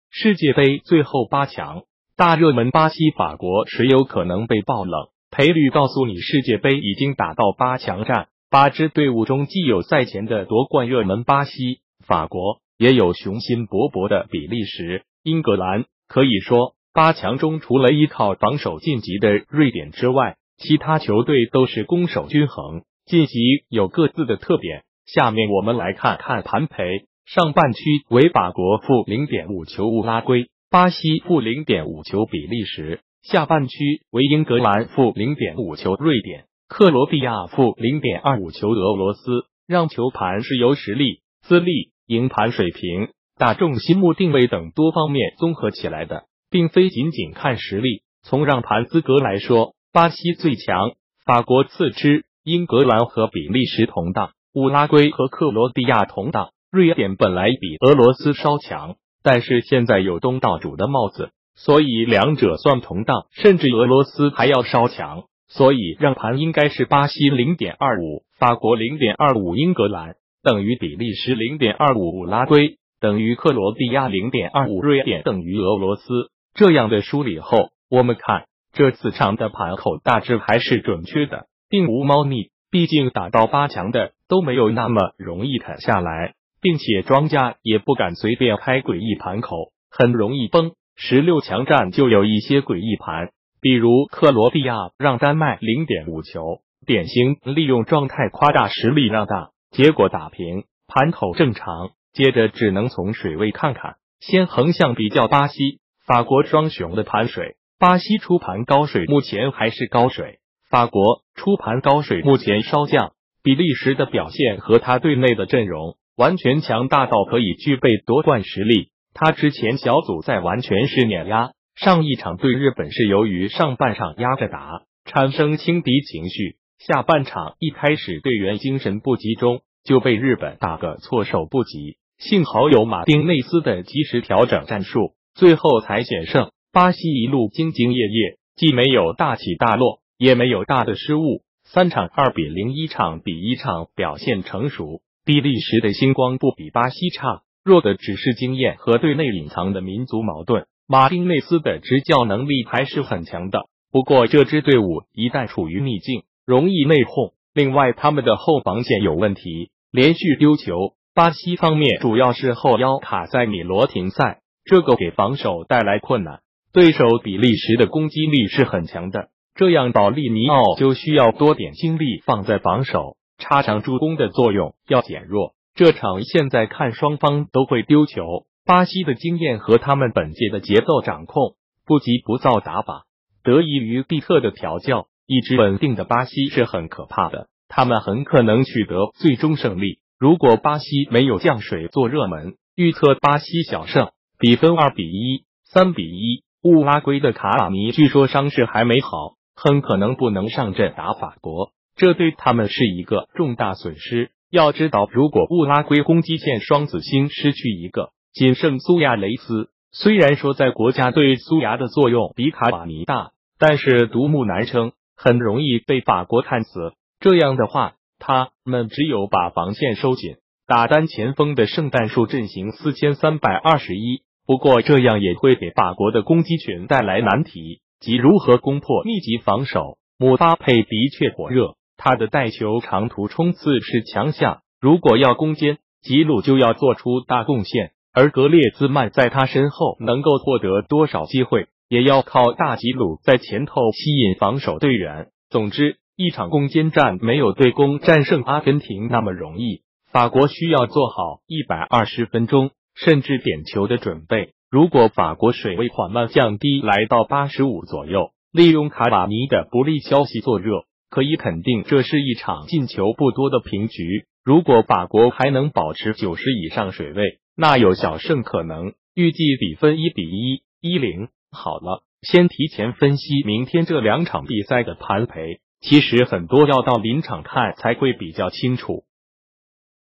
世界杯最后八強，大熱門巴西法國誰有可能被爆冷，賠率告訴你
8支隊伍中既有賽前的奪冠熱門巴西、法國，也有雄心勃勃的比利時、英格蘭，可以說，八強中除了依靠防守晉級的瑞典之外，其他球隊都是攻守均衡。